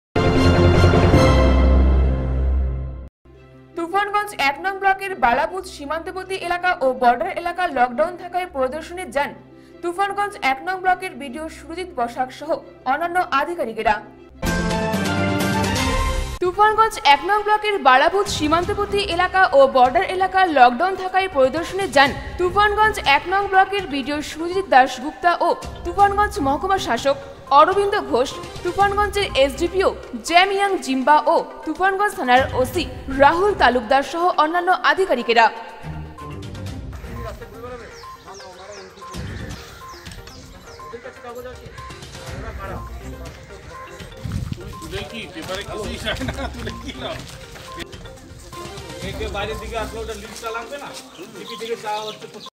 বালাভূত সীমান্তবর্তী এলাকায় ও বর্ডার এলাকা পরিদর্শনে তুফানগঞ্জের বিডিও সহ আধিকারিকরা
তুফানগঞ্জ ১নং ব্লকের বালাভূত সীমান্ত বর্তী এলাকায় ও বর্ডার এলাকা লকডাউন থাকায় পরিদর্শন করলেন তুফানগঞ্জ ১নং ব্লকের বিডিও সহ অন্যান্য অধিকারিকরাও